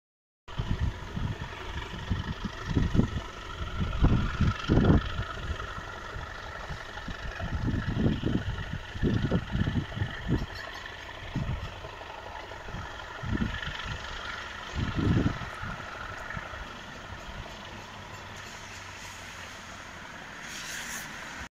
NYMR Santa Specials Lambton tank no 29 and class 37 leaving Goathland
No 29 Lambton tank leaving Goathland with a Santa Special.